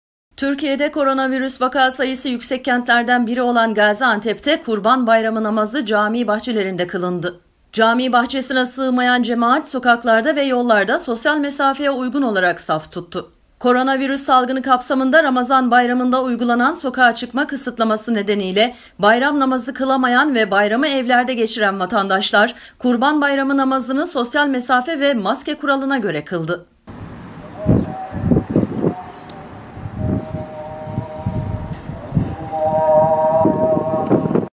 GAZİANTEPLİLER BAYRAM NAMAZINDA SOKAKLARA TAŞTI
Türkiye’de korona virüs vaka sayısı yüksek kentlerden biri olan Gaziantep’te Kurban Bayramı namazı cami bahçelerinde kılındı.
Türkiye'de korona virüs vaka sayısı yüksek kentlerden biri olan Gaziantep'te Kurban Bayramı namazı cami bahçelerinde kılındı. Cami bahçesine sığmayan cemaat, sokaklarda ve yollarda sosyal mesafeye uygun olarak saf tuttu.